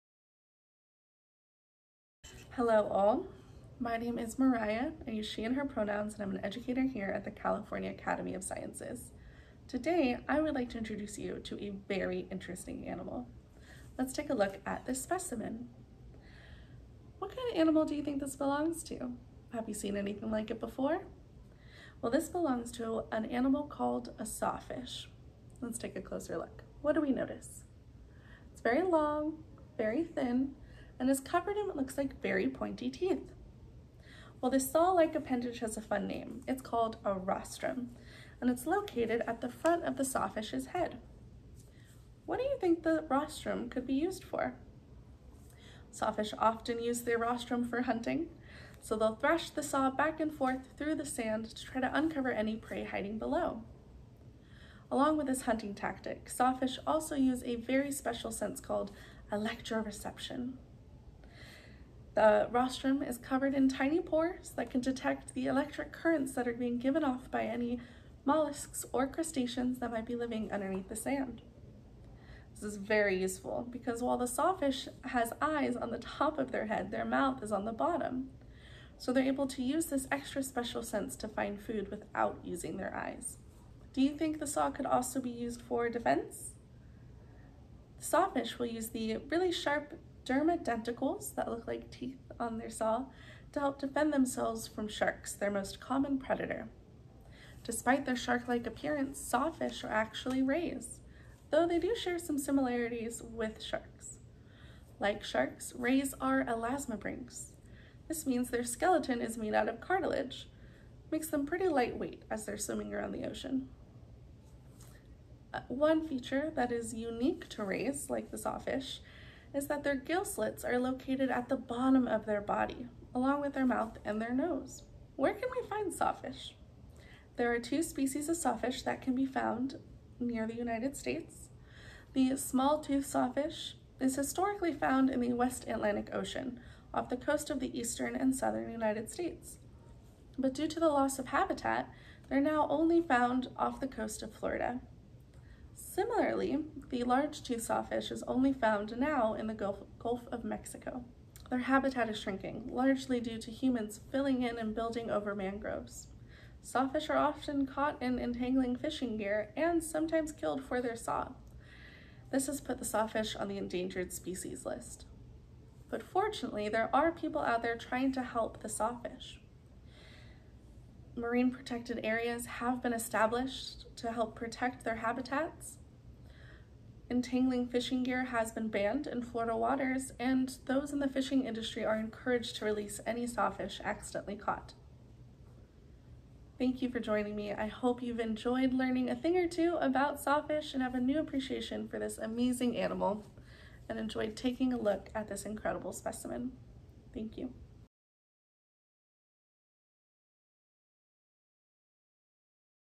Sawfish Specimen Spotlight | California Academy of Sciences
With a body like a shark, and a face like a carpenter’s saw… this unusual family of rays never fails to amaze. Join Academy Educator Moriah to get to know the sawfish through this short video from the Academy’s Naturalist Center!

The Academy’s critical work continues even while our museum doors remain temporarily closed. — any amount helps. Thank you!